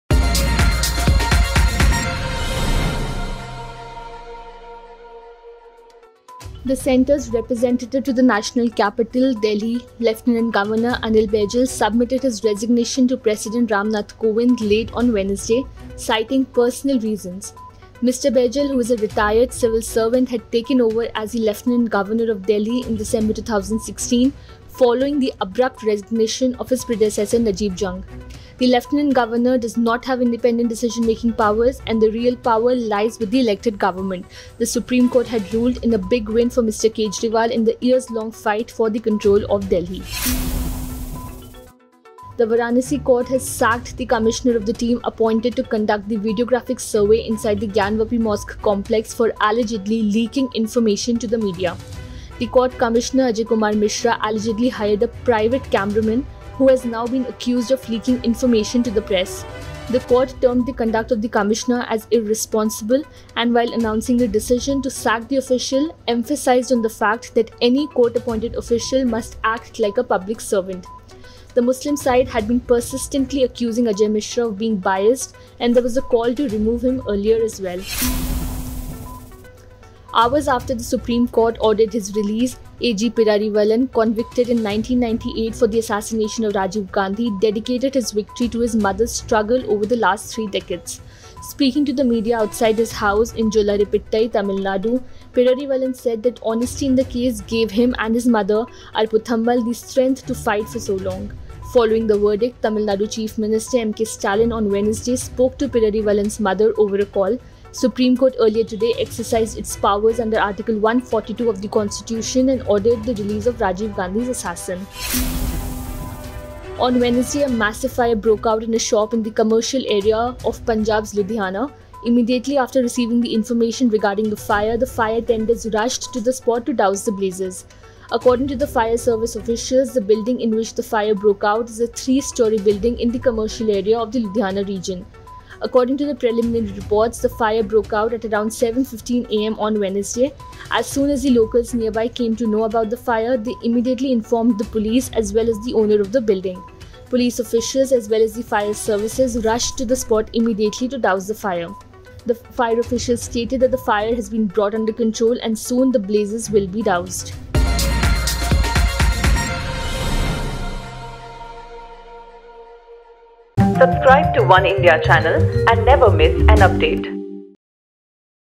Delhi Lieutenant Governor Anil Baijal submits his resignation | OneIndia News
Delhi Lieutenant Governor Anil Baijal submits his resignation; Official appointed by court to survey Gyanvapi Mosque axed; CM Stalin speaks to Perarivalan’s mother after Supreme Court verdict; Fire breaks out at a shop in Ludhiana